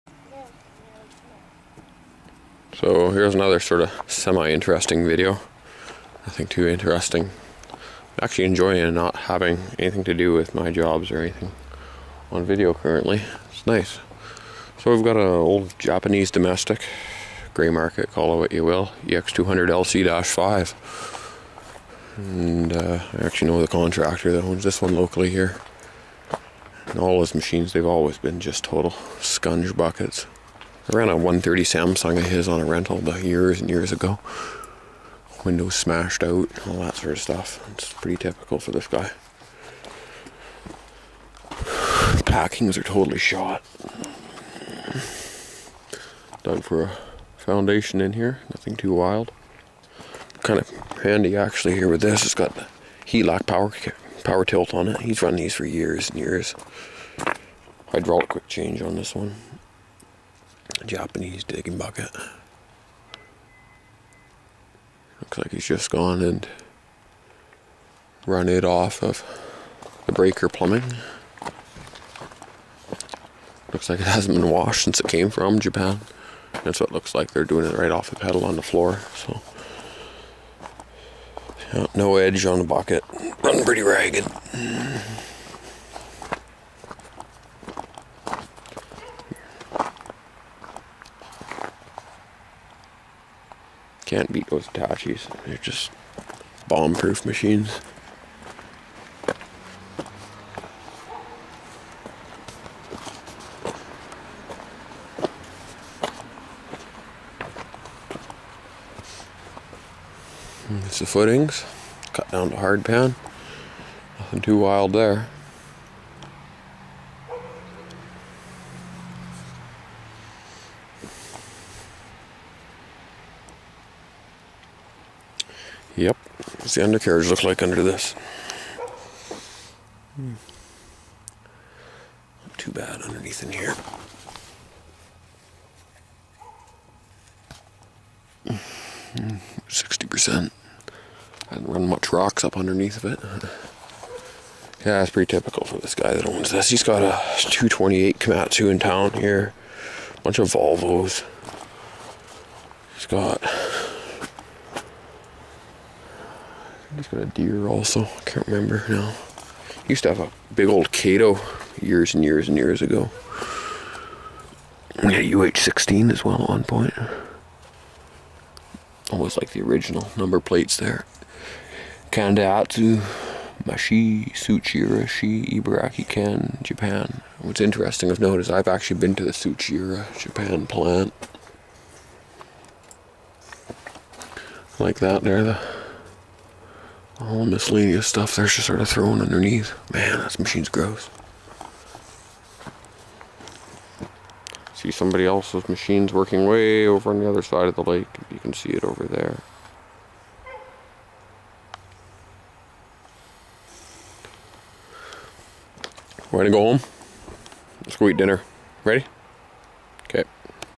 A filthy Hitachi EX200LC-5 excavator with Helac tilt & hyd. quick-change.
A semi-interesting video of one of the grimier excavators I have been around. A Japanese domestic model EX200LC-5. This has a Helac tilter and a hydraulic quick change.